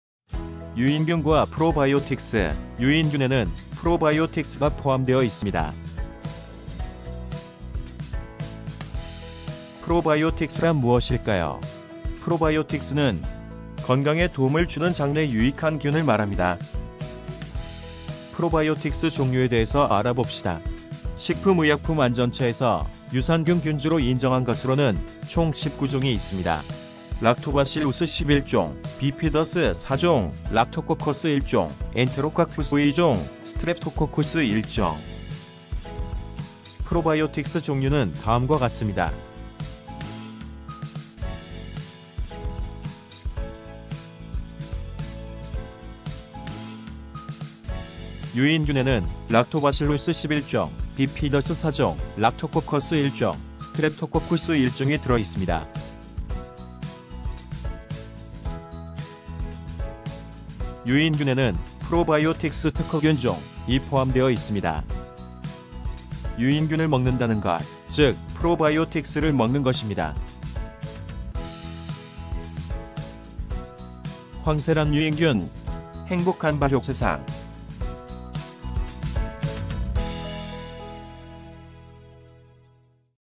유인균과 프로바이오틱스
유인균을 먹으면, 프로바이오틱스를 먹지 않아도 되나요? 식품의약품안전처에서 인정한 유산균 균주(고시형) 프로바이오틱스 + 특허유산균이 포함되어 있습니다.